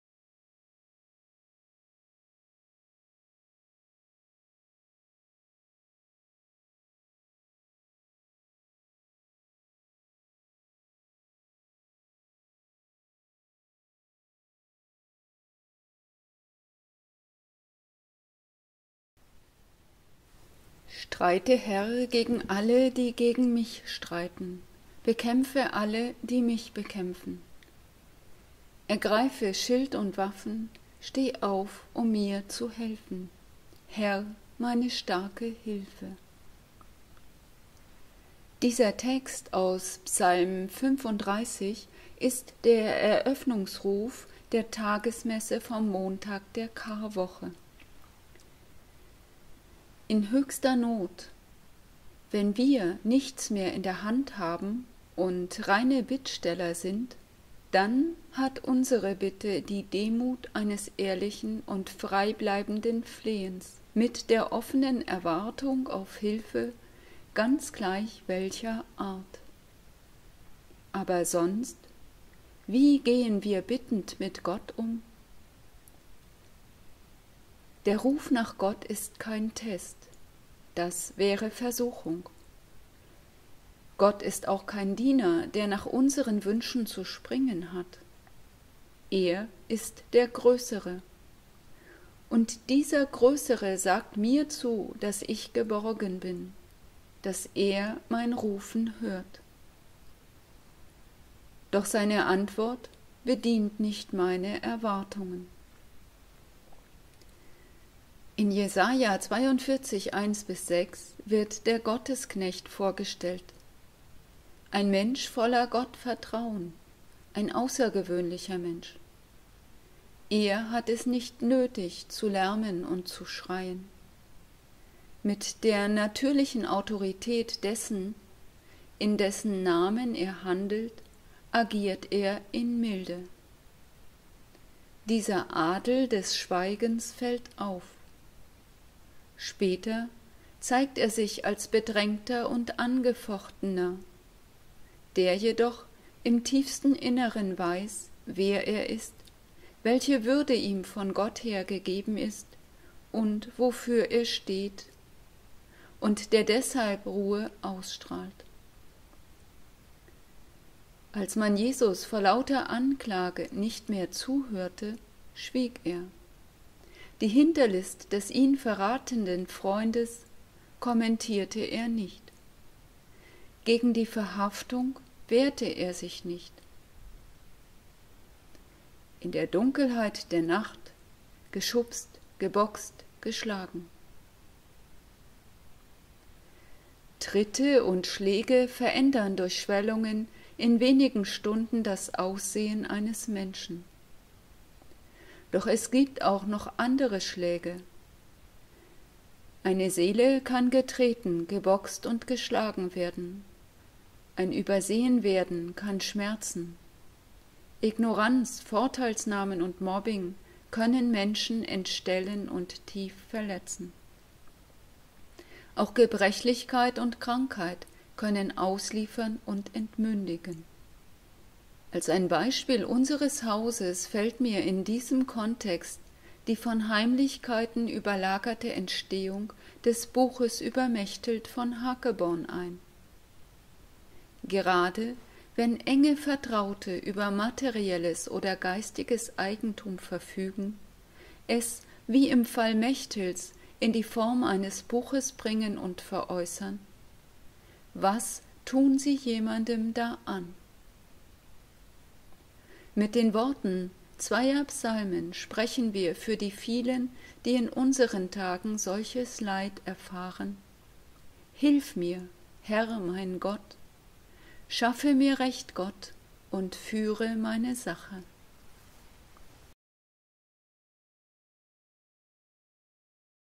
Montag der Karwoche - Impulse aus dem Kloster Helfta (15)
Meditation zum Eröffnungsvers der Tagesmesse am Montag in der Karwoche:

Steh auf, Herr, um mir zu helfen!

Gott ist der Größere. Und Er sagt mir zu, dass ich geborgen bin. Selbst wenn Er nicht so handelt, wie ich es mir wünsche.

In Zeiten, in denen ich Leid und Unrecht erfahren muss, darf ich auf Gottes Hilfe hoffen und Ihn anrufen.